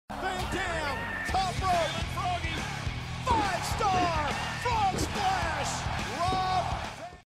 2013 WWE Rob Van Dam Finisher "Five Star Frog Splash"
Five Star Frog Splash (High-angle frog splash, sometimes while turning position in mid-air)